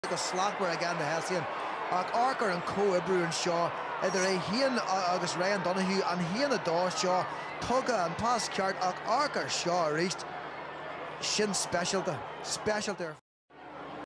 Evasion skills : The side step
As you do with a “roll off” an effective evasive side step starts with a solo. You go into the movement with a solo so you can take your steps then do your bounce if required.